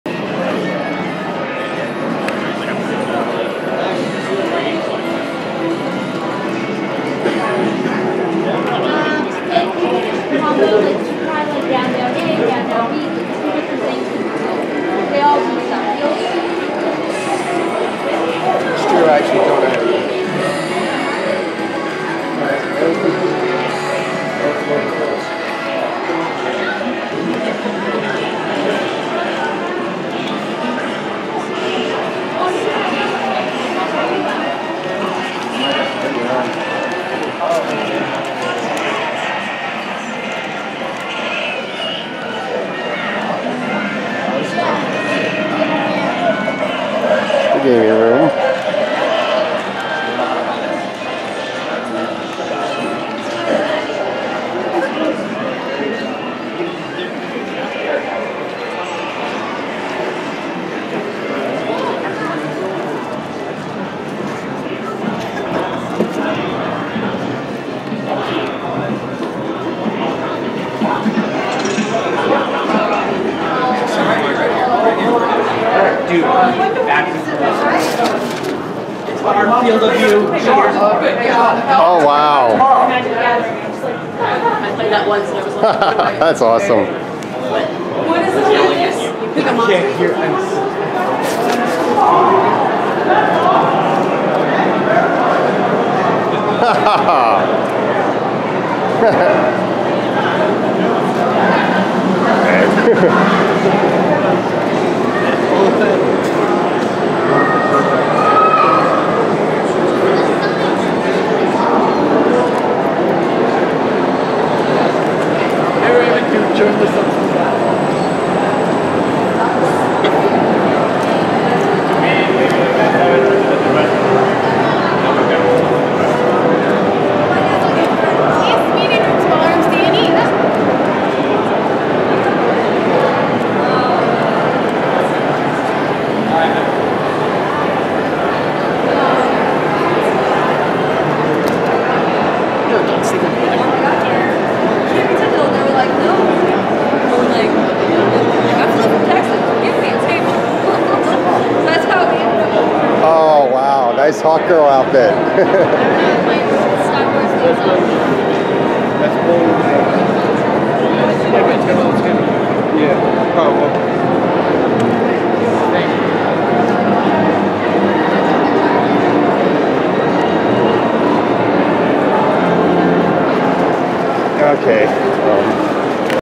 RI Comic con 2016 camera footage Pt 8
I deleted pt 7 because it was too short and it didn't have anything worth watching. Here is more footage from Rhode island Comic con